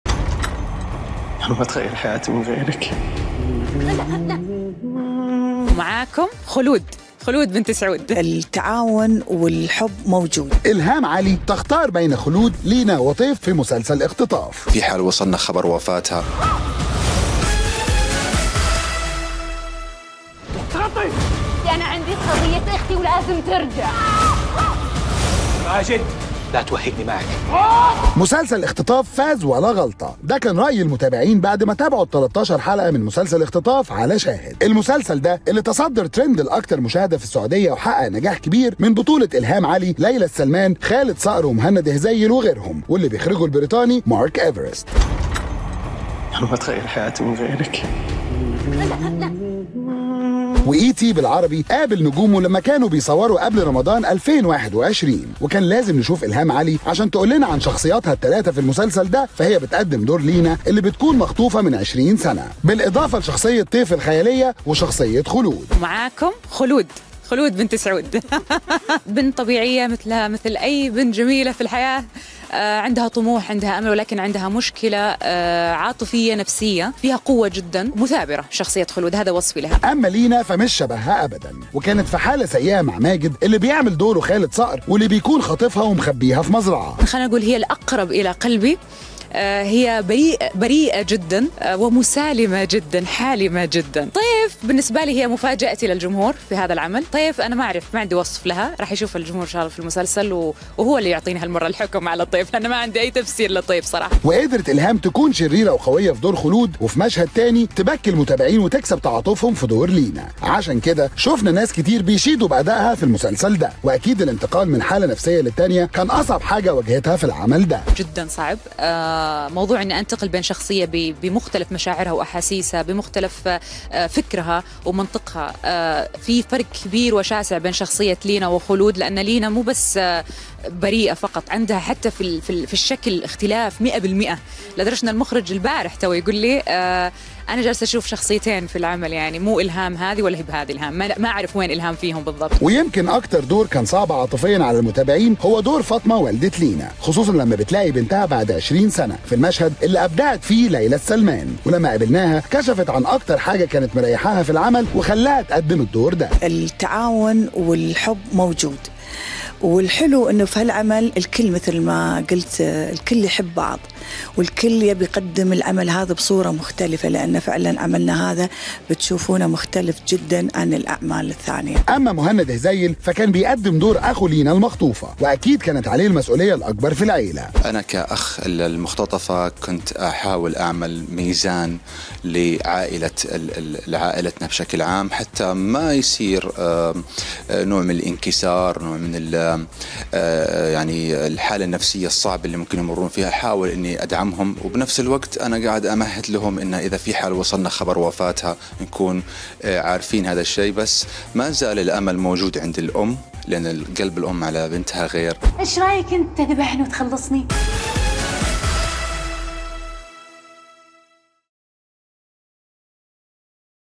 إلهام علي تختار بين خلود، لينا وطيف في مسلسل "إختطاف"🎬
"مسلسل اختطاف فاز ولا غلطة" هذا كان رأي المتابعين بعدما تابعوا الـ13 حلقة من مسلسل "إختطاف" على منصة شاهد، حيث تصدر المسلسل الترند الأكثر مشاهدة في السعودية وحقق نجاح كبير، وهو من بطولة إلهام علي، ليلى السلمان، خالد صقر، مهند هذيل وغيرهم ومن إخراج البريطاني Mark Everest.

ET بالعربي كان التقى مع نجومه عندما كانوا يصورون العمل قبل رمضان 2021، وكان من الضروري أن نلتقي إلهام علي لتخبرنا عن شخصياتها الثلاثة بهذا المسلسل فهي بتقدم  دور "لينا" المخطوفة منذ 20 سنة، بالإضافة لشخصية طيف الخيالية، وشخصية خلود.

كما إلتقى ET بالعربي، بـ ليلى السلمان ومهند هذيّل حيث تحدثوا عن كواليس العمل وشخصياتهم.